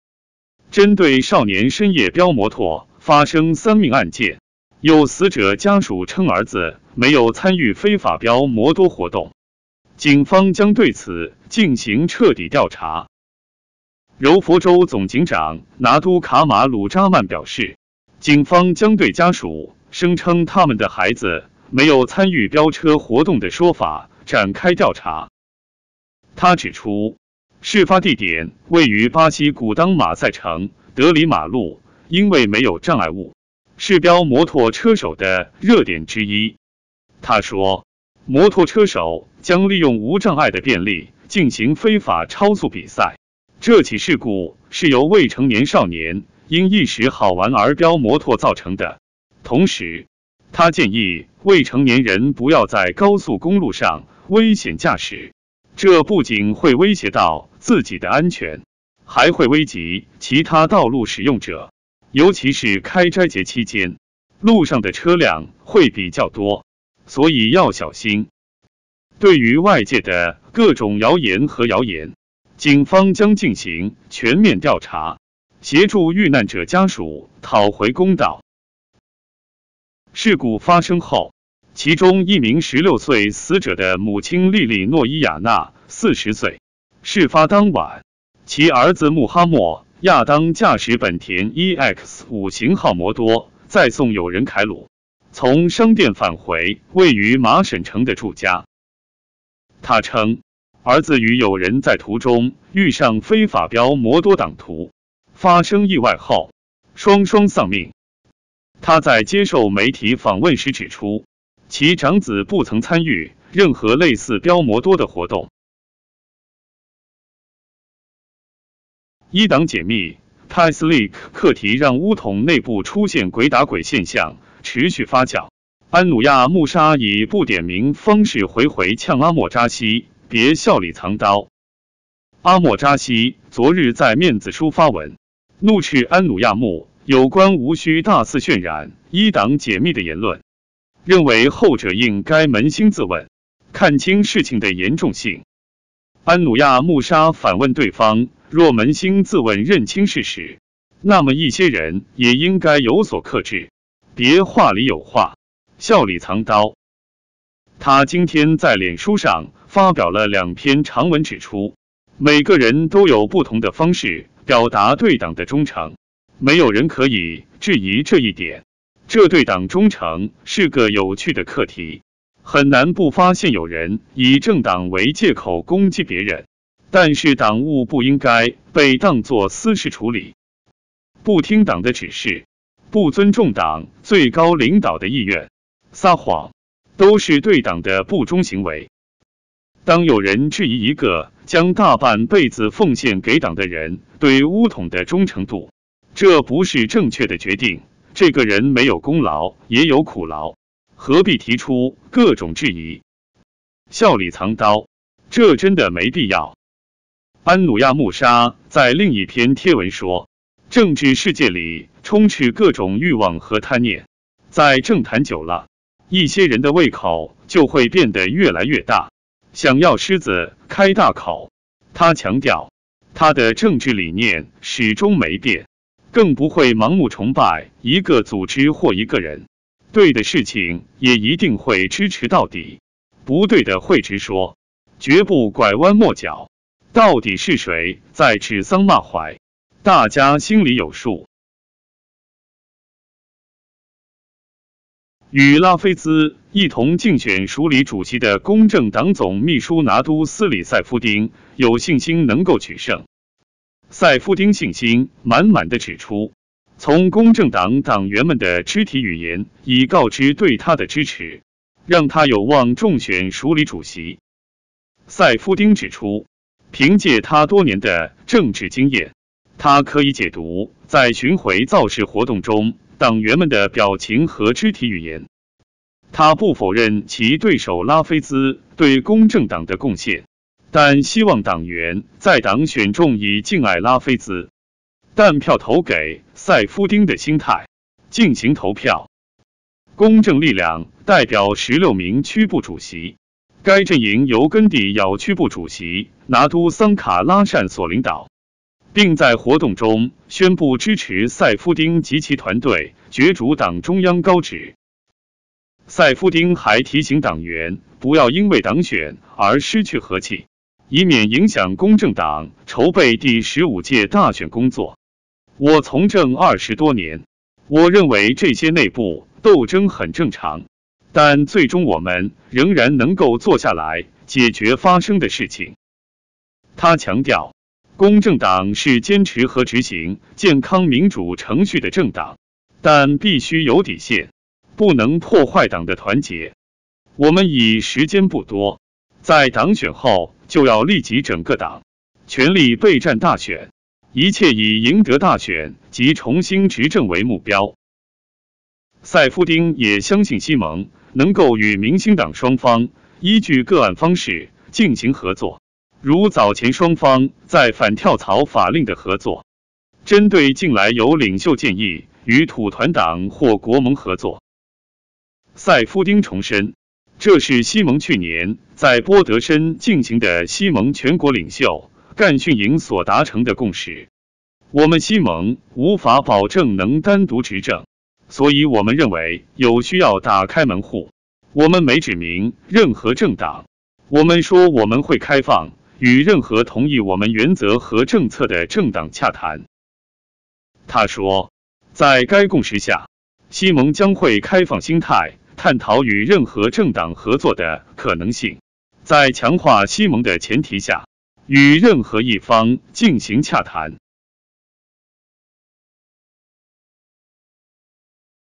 警方开始行动了！飚摩托死者母亲称儿子没参与非法飚摩托一事，柔佛州总警长表示，警方将进行全面调查，协助遇难者家属讨回公道。
针对少年深夜飙摩托发生3命案件，有死者家属称儿子没有参与非法飙摩哆活动，
警方将对此进行彻底调查。
柔佛州总警长拿督卡玛鲁扎曼表示，警方将对家属声称他们的孩子没有参与飙车活动的说法展开调查。
他指出，事发地点位于巴西古当马赛城德里马路因为没有障碍物，是飙摩托车手的热点之一。